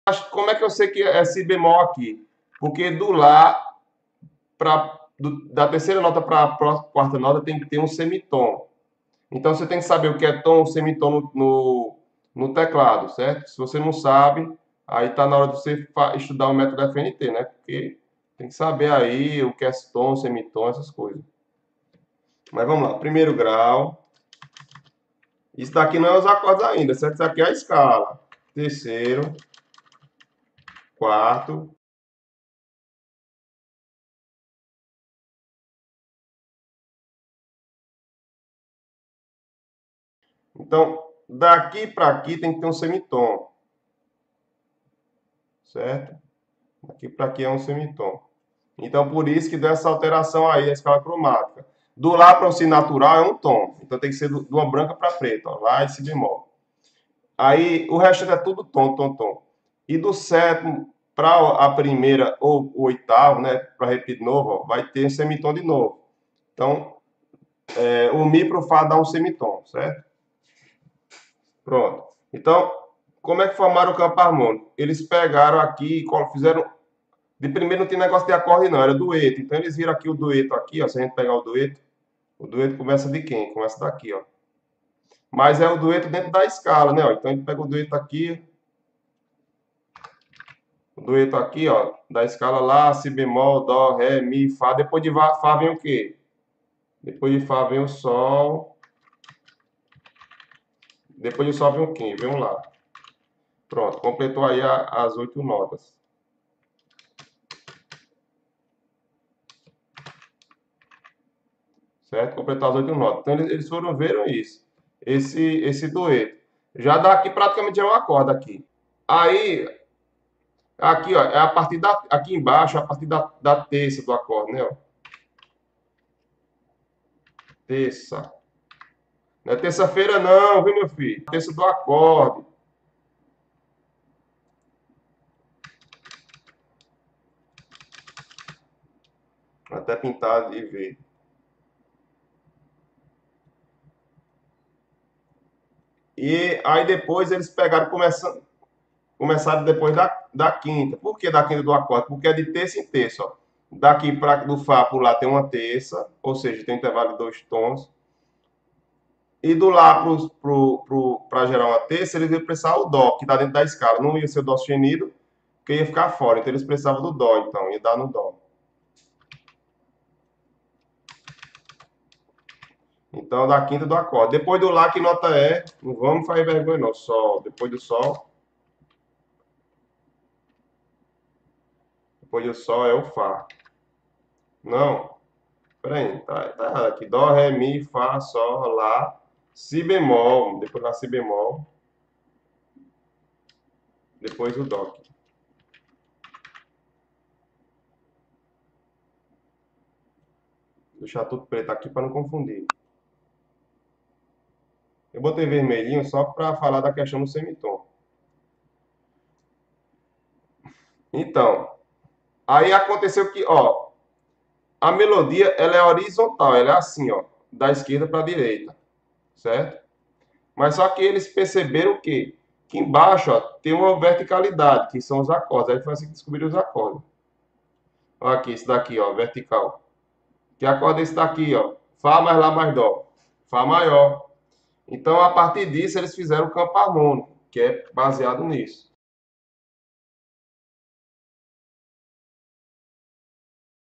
👨🏽‍🎓🎵Teoria - Como Formar a escala maior de Fá e seu campo harmônico maior - Ricardo Brito
Fala galera, aqui está o link do nosso site oficial para comprar ritmos, cursos ou entrar em contato